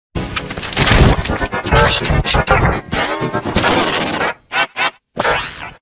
Klasky Csupo In G Major 937 (Instructions In Description)
Note 2: This effect was made in VEGAS Pro on November 30, 2018

How to make G Major 937:

(Video FX)

1st: HSL Adjust - Invert Color

2nd: LAB Adjust - Invert Luminosity

3rd: Pinch/Punch - Medium Punch

4th: Mirror - Reflect Bottom

5th: Wave - Large

(Audio FX)

elastique
Pro

-9
-5
2
9
12

Effect by SRGFWD980